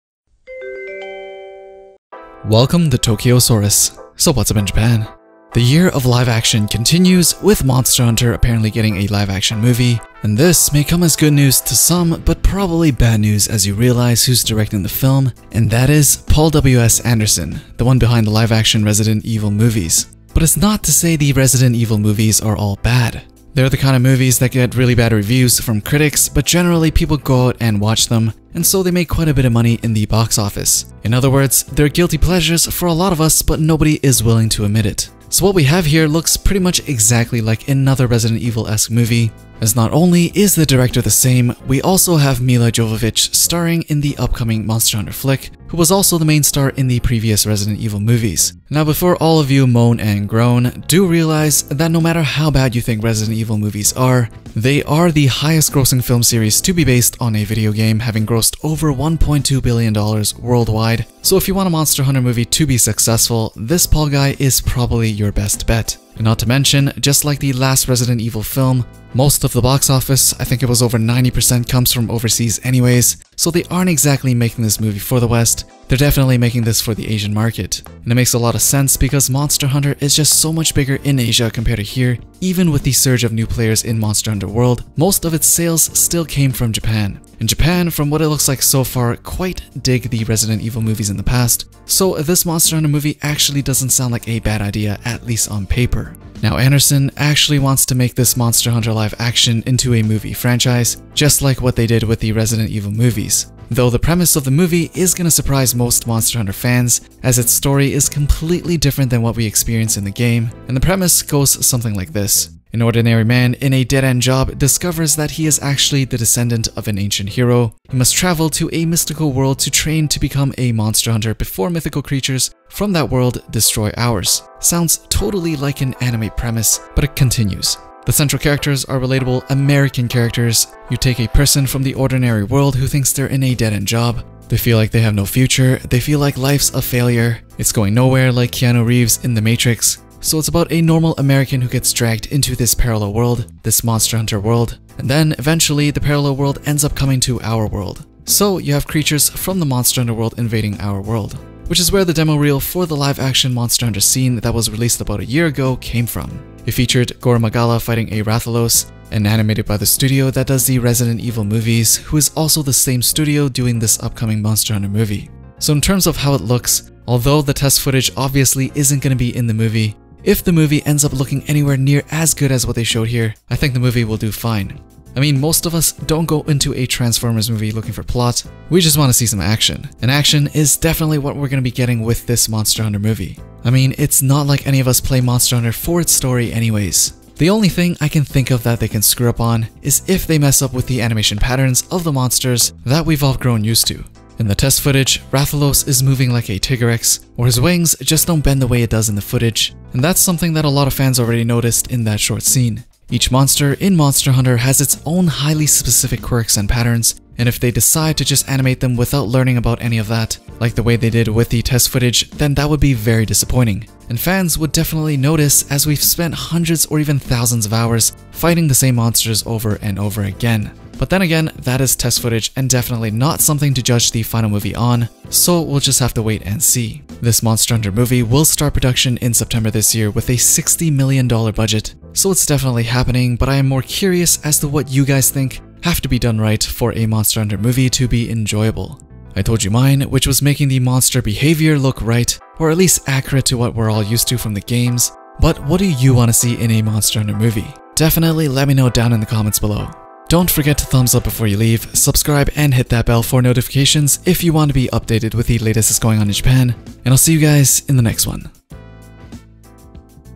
Live Action Monster Hunter Movie Looks...
A live action Monster Hunter movie is confirmed to start production in September later this year and will be directed by Paul WS Anderson the director behind the live action Resident Evil movies. How will Monster Hunter's hardcore fanbase respond to a hollywood's take on the beloved franchise? Will a good action be good enough to satisfy its fans? The Resident Evil movie franchise is the most successful video game movies in terms of box office but can it do the same with Monster Hunter? They are planning on making Monster Hunter into a movie series as well, so do expect more to come after the first one.